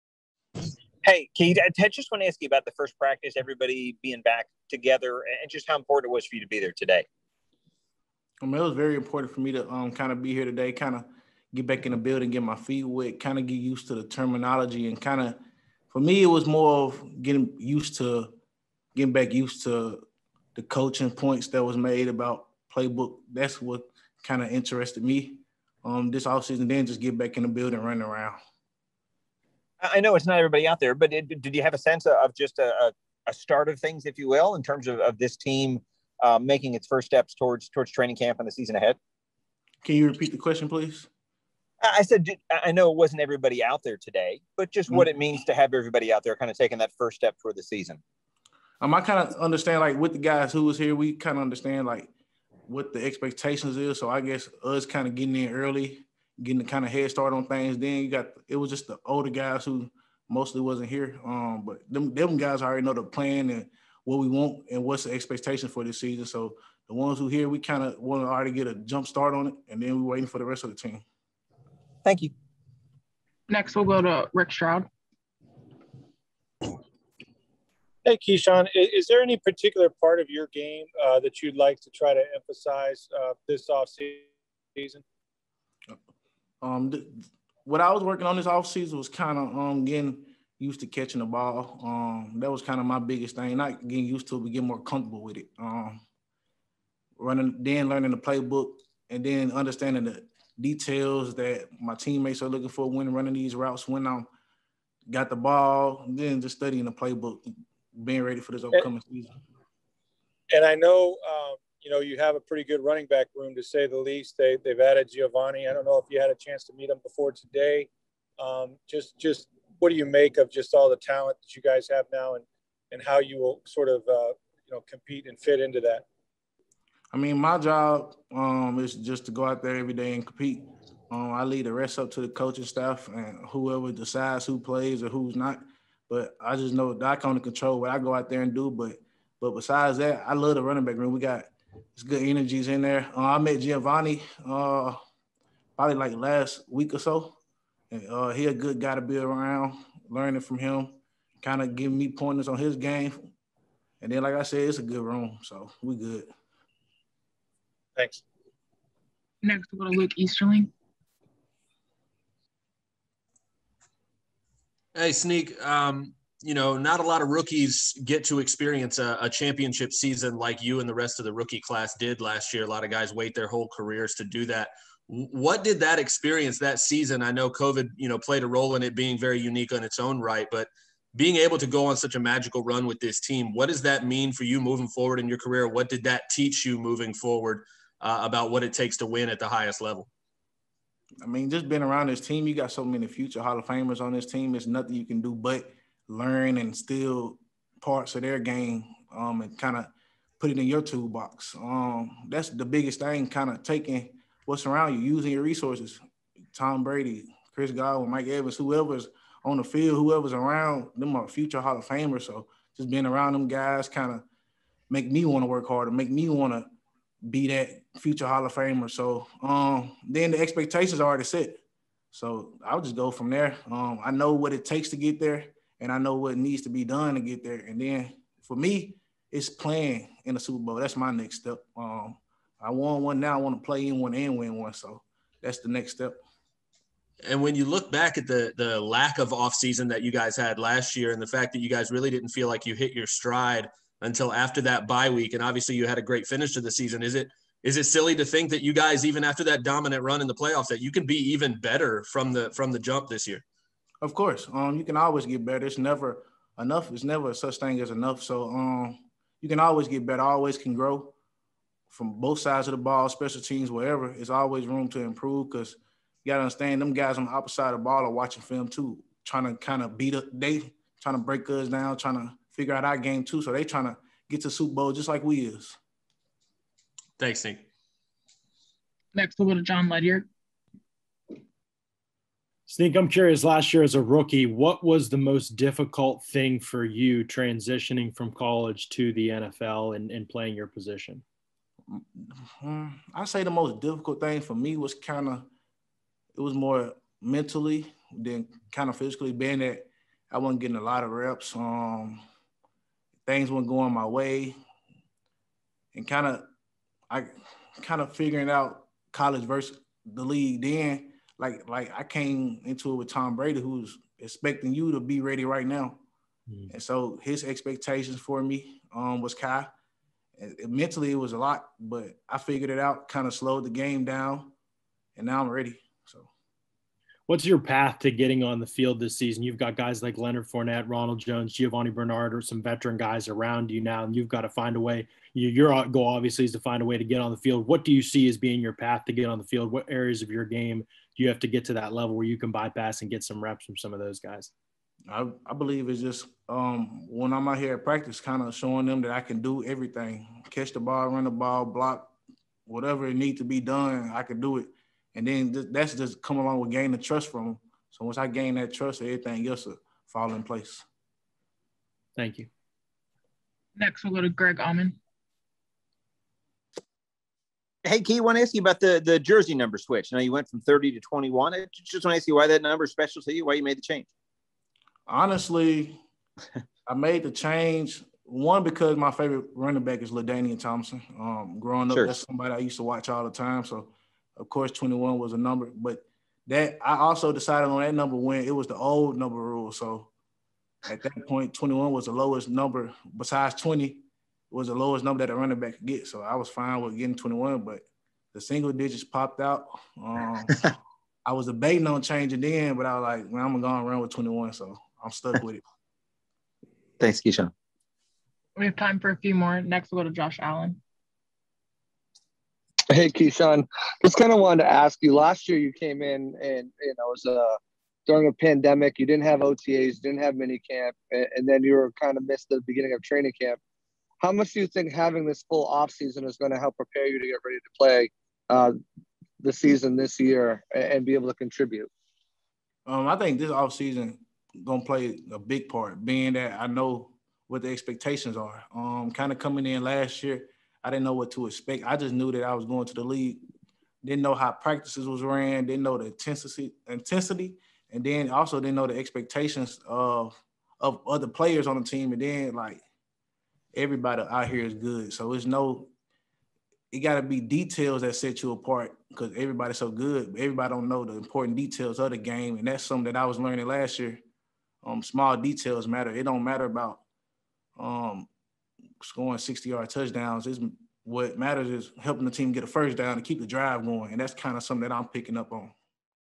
Ke'Shawn Vaughn on Playing with Giovani Bernard, Preparing for Year Two | Press Conferences
Buccaneers running back Ke'Shawn Vaughn spoke to the media on Tuesday following the first day of OTA's.